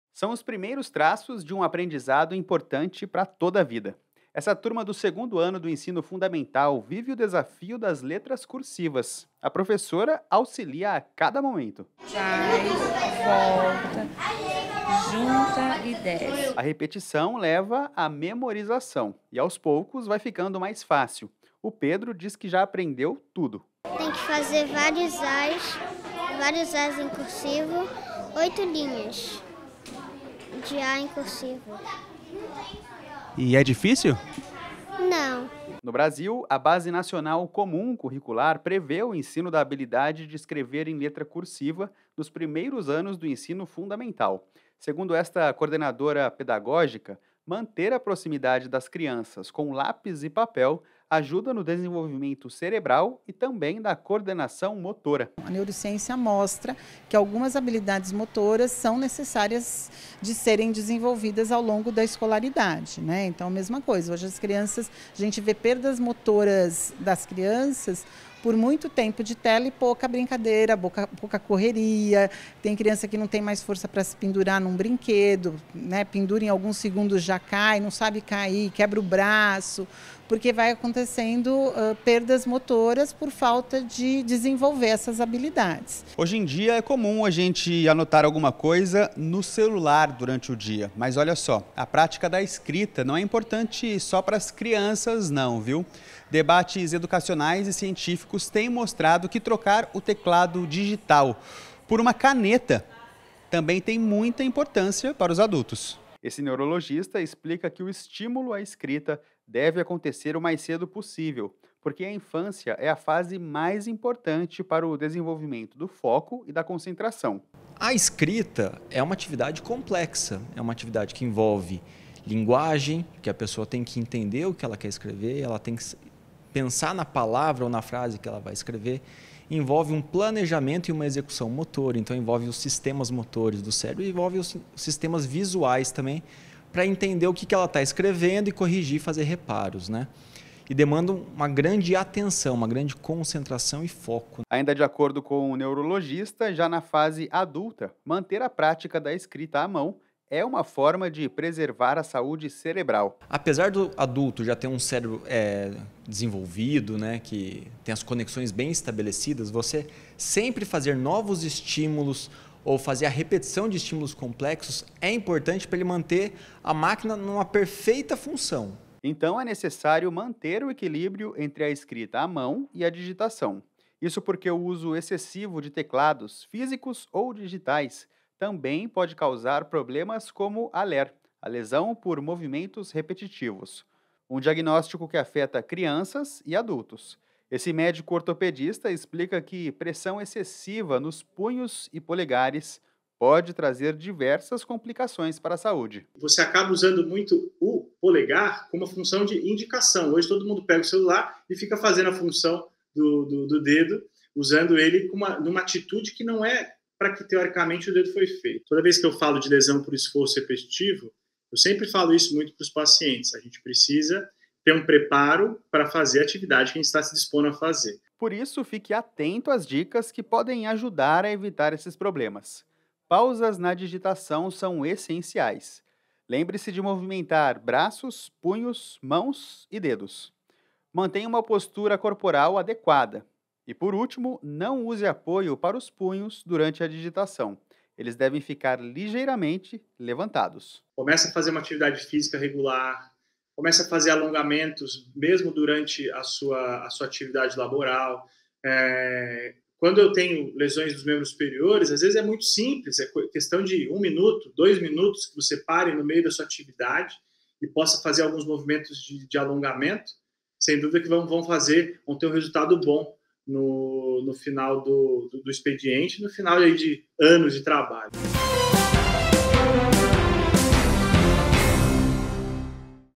BENEFÍCIOS DA ESCRITA A MÃO
Na era digital, a palavra "escrever" foi trocada por "digitar"... Colocar palavras no papel tá cada vez mais longe da rotina de muita gente! Mas pessoal, escrever a mão tem benefícios viu! Aumenta a concentração, a memória... Aprendizagem... E ainda protege de doenças como l.e.r. E tendinite, causada por esforços respetivos com uso de teclado e mouse!